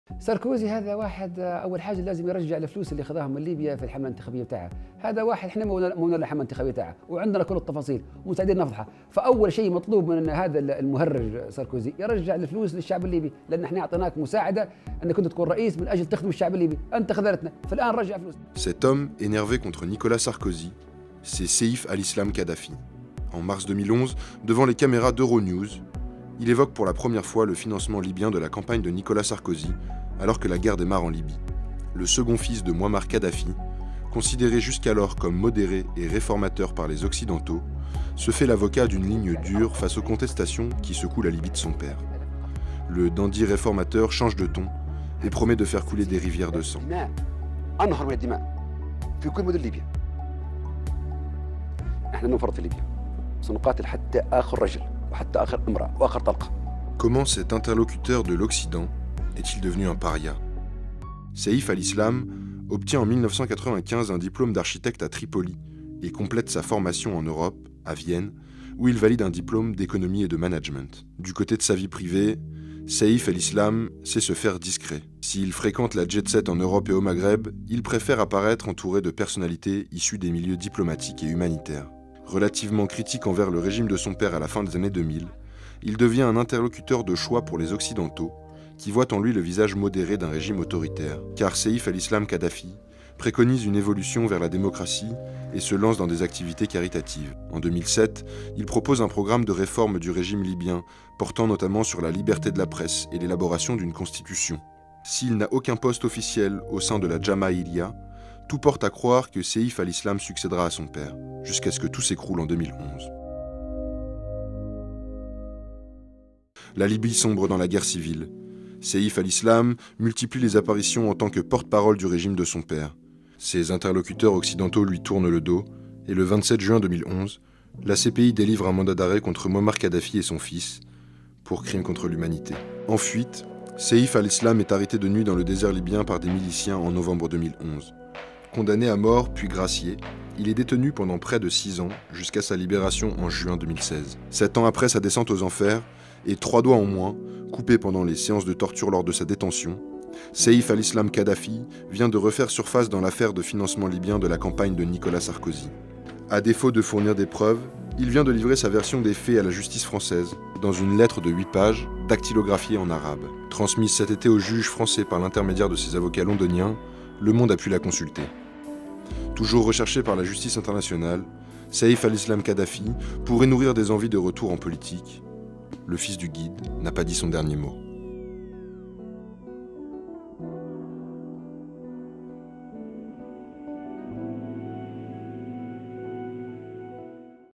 Saïf Al-Islam Kadhafi, le modéré devenu paria
Le fils de Mouammar Kadhafi a transmis à la justice française un courrier dans lequel il affirme à nouveau que la campagne de Nicolas Sarkozy a été financée par la Libye. C'est par lui qu'avait éclaté le scandale en 2011.

Après la chute du "Guide", Saïf Al-Islam Kadhafi a passé six longues années en détention. Portrait d'un modéré devenu un paria suite à ses prises de position au début de la guerre en Libye.